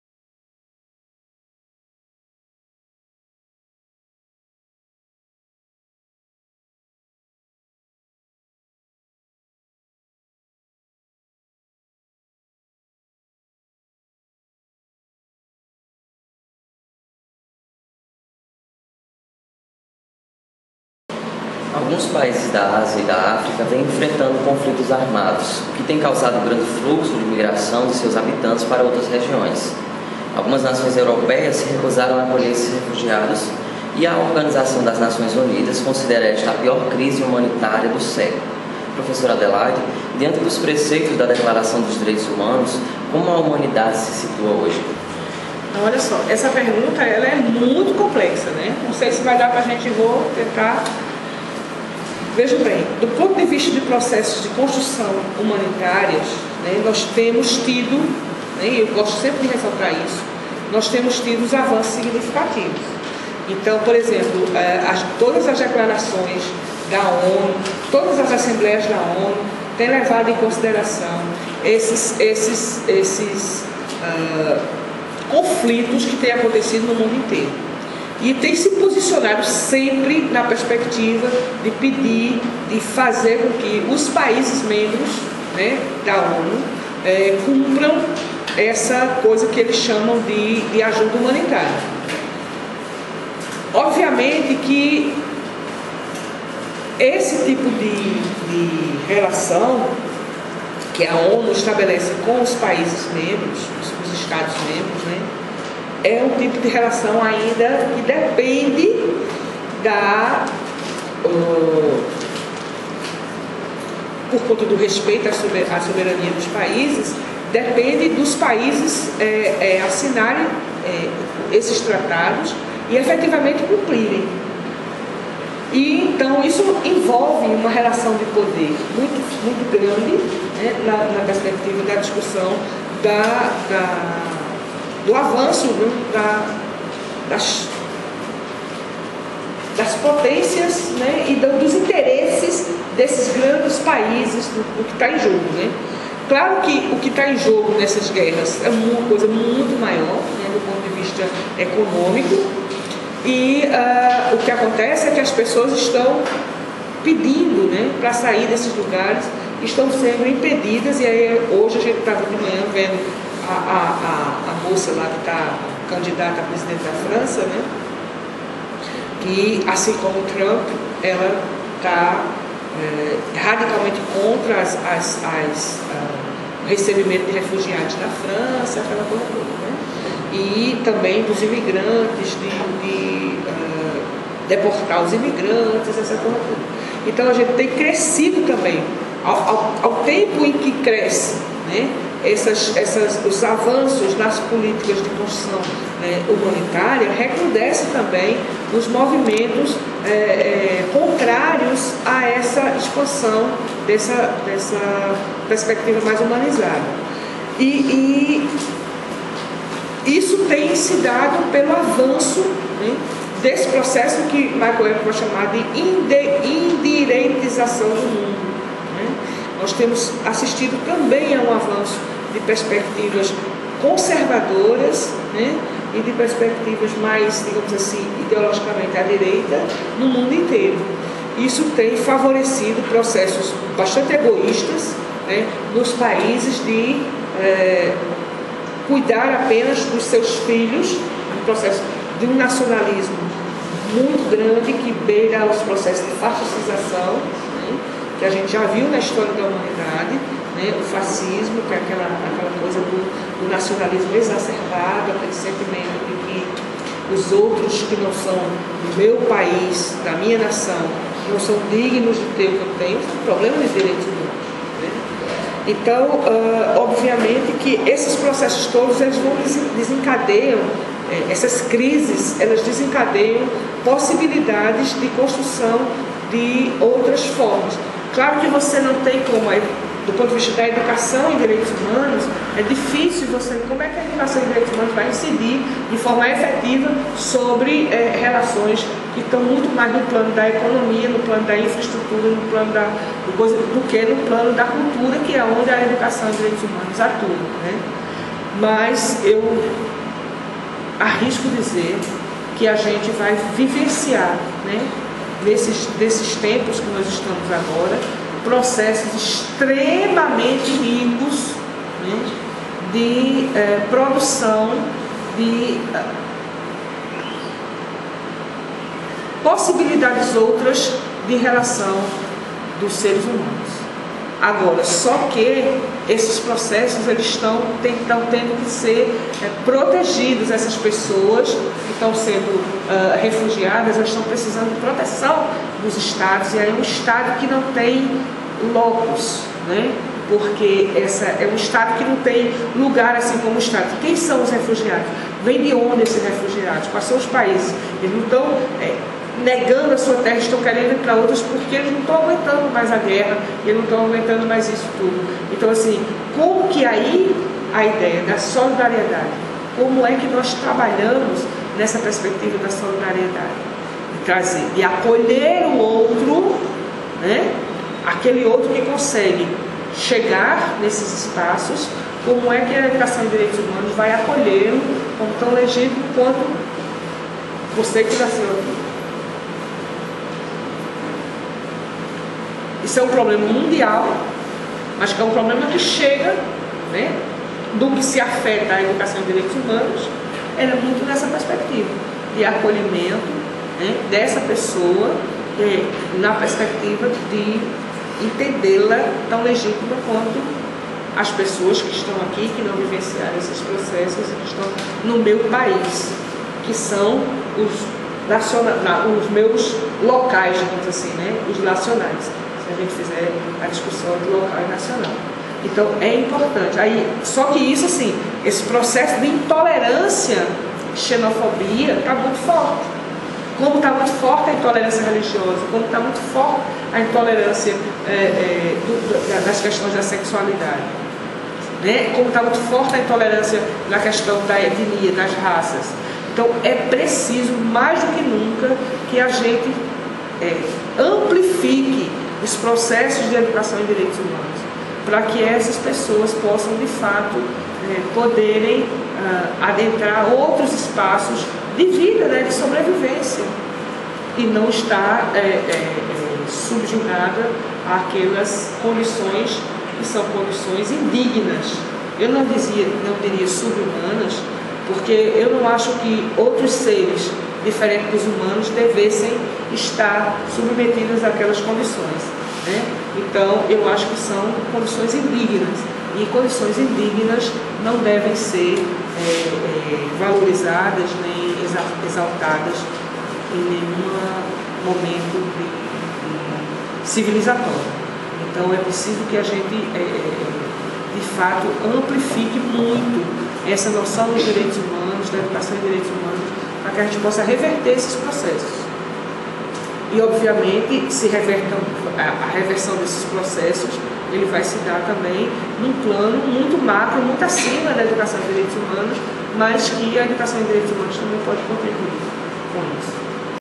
Vídeo 7 - V Colóquio Fundamentos dos Direitos Humanos com Adelaide Alves Dias
7 - Crises humanitárias e relações entre países
Aqui a professora faz um panorama global dos Direitos Humanos levando em conta a crise de refugiados, valores humanitários e o avanço do conservadorismo.
- 7º vídeo do V Colóquio de Fundamentos dos Direitos Humanos, realizado no dia 23 de março de 2017 com a professora Adelaida Alves Dias (Coordenadora do Programa de Pós-Graduação em Direitos Humanos, Cidadania e Políticas Públicas da UFPB), organizado por Allan Nunes, Anderson Santana, Bruno Marinho e Gustavo Alves. Supervisão: Carmélio Reynaldo Ferreira.